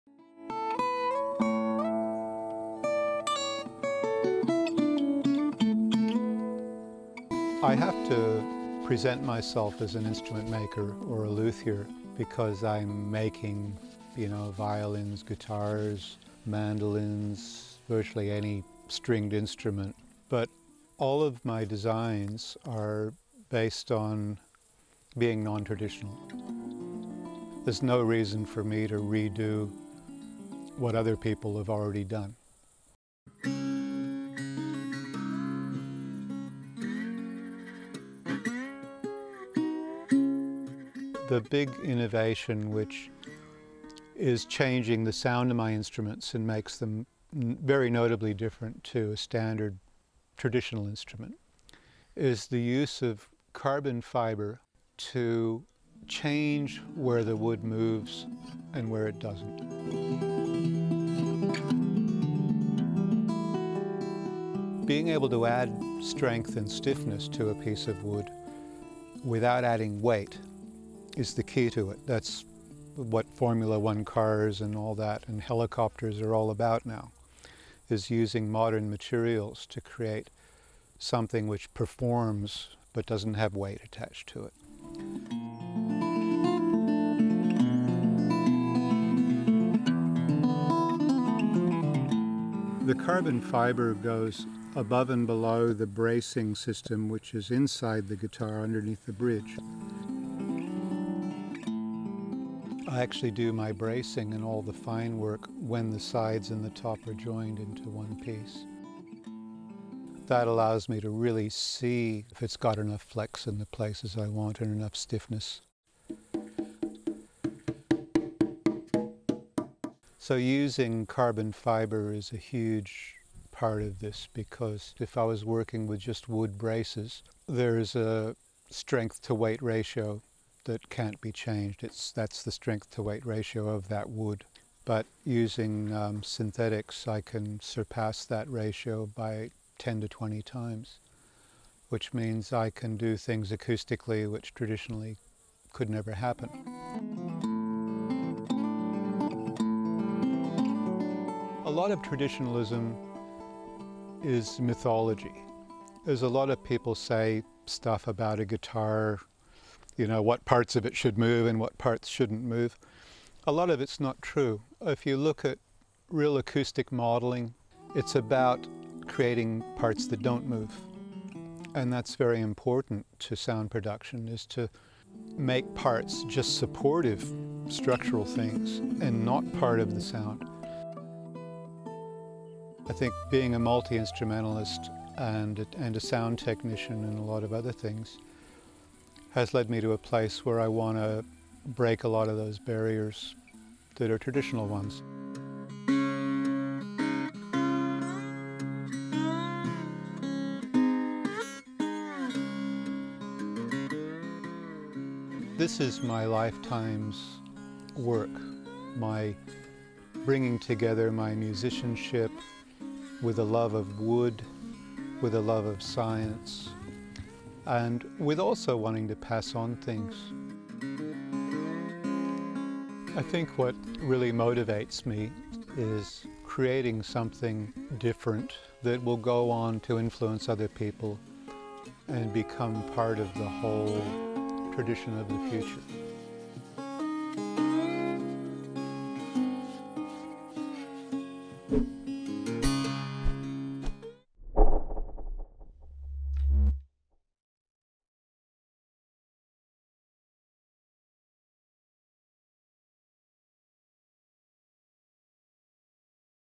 The science of instrument-making | Creators | ABC Australia
Do you think of instrument-making as an old-world craft?
Well, it doesn’t need to be, as Daniel Brauchli’s work goes to show. He’s using science and innovative design to bust the myths that surround instrument construction.

A luthier is someone who builds or repairs string instruments generally consisting of a neck and a soundbox.
The word "luthier" comes from the French word luth, which means lute. A luthier was originally a maker of lutes, but the term now includes makers of stringed instruments such as the violin or guitar.

Produced by:
Sarah Abbott

Transcript:

I have to present myself as an instrument maker or a luthier because I'm making, you know, violins, guitars, mandolins.
Virtually any stringed instrument.
But all of my designs are based on being non-traditional.
There's no reason for me to redo what other people have already done.
The big innovation, which is changing the sound of my instruments,
and makes them very notably different to a standard traditional instrument
is the use of carbon fiber to change where the wood moves, and where it doesn't.
Being able to add strength and stiffness to a piece of wood without adding weight is the key to it.
That's what Formula One cars and all that and helicopters are all about now.
Is using modern materials to create something which performs but doesn't have weight attached to it.
The carbon fiber goes above and below the bracing system,
which is inside the guitar underneath the bridge.
I actually do my bracing and all the fine work when the sides in the top are joined into one pace.
That allows me to really see if it's got enough flex and the places I want and enough stiffness.
So using carbon fiber is a huge part of this because
if I was working with just wood braces, there is a strength to weight ratio,
that can't be changed. It's that's the strength to weight ratio of that wood.
But using synthetics, I can surpass that ratio by 10 to 20 times.
Which means I can do things acoustically which traditionally could never happen.
A lot of traditionalism is mythology.
There's a lot of people say stuff about a guitar.
You know what parts of it should move and what parts shouldn't move.
A lot of it's not true. If you look at real acoustic modeling.
It's about creating parts that don't move.
And that's very important to sound production, is to make parts just supportive structural things
and not part of the sound.
I think being a multi-instrumentalist, and a sound technician and a lot of other things.
Has led me to a place where I want to break a lot of those barriers that are traditional ones.
This is my Lifetime's work,
my bringing together my musicianship with a love of wood
with a love of science and with also wanting to pass on things.
I think what really motivates me is creating something different that will go on to influence other people
and become part of the whole traditional education.